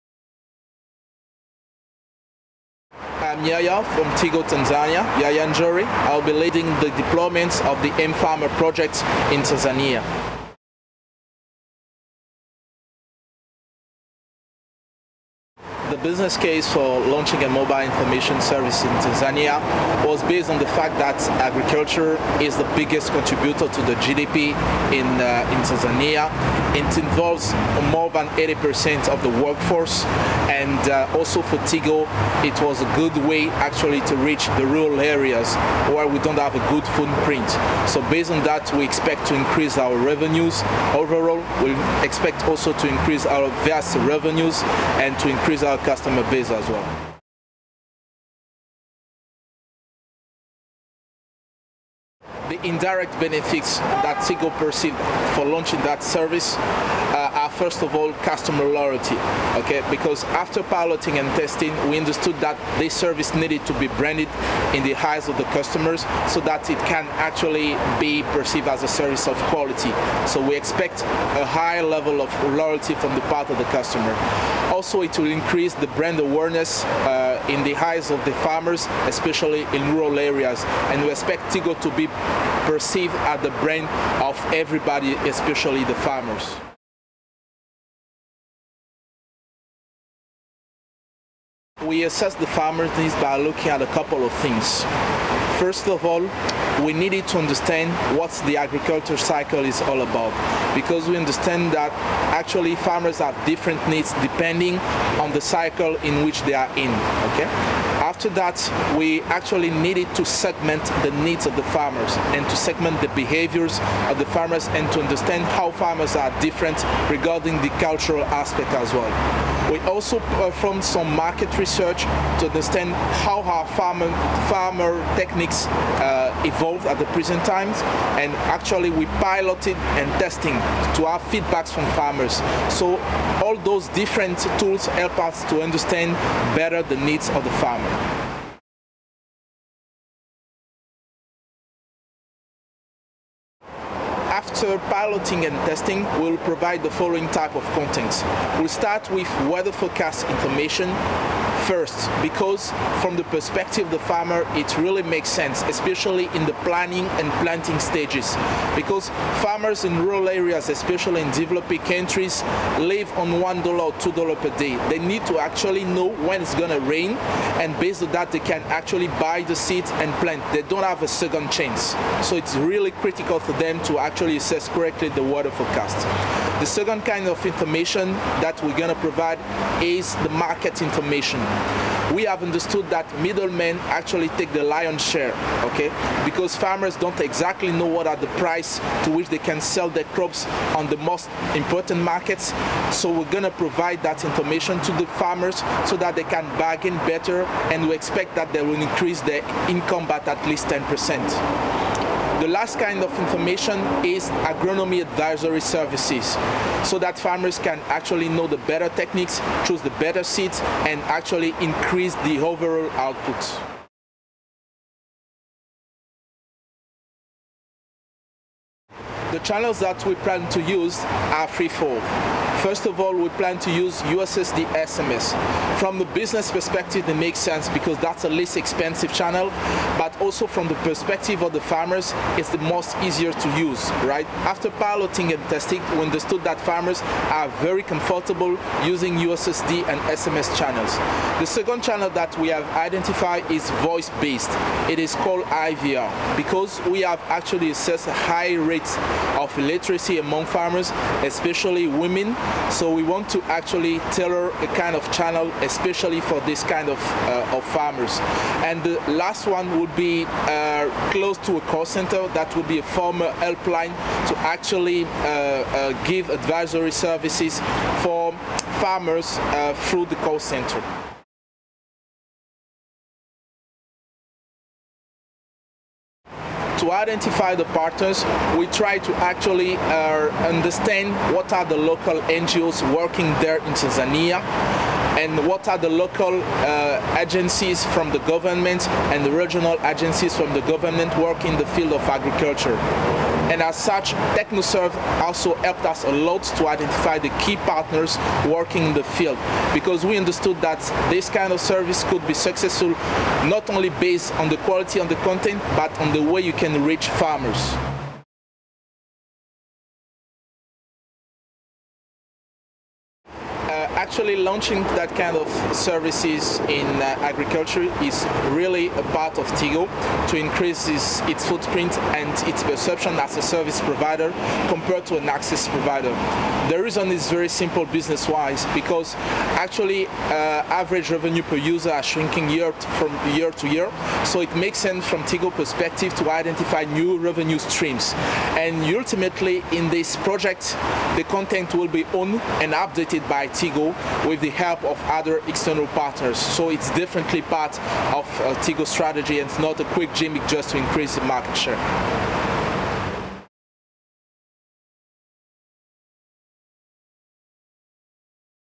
Interview with Tigo Tanzania on launching an Agri VAS
In this video, Yaya N'djore, the mFarmer Project Manager at Tigo Tanzania, discusses the mobile agricultural information service Tigo will launch for farmers in Tanzania. Areas covered include the business case and indirect benefits for Tigo, the approach to agricultural content and assessing farmers' information needs, the types of content to be delivered and the channels used, partnerships and Tigo's strategy as a service provider.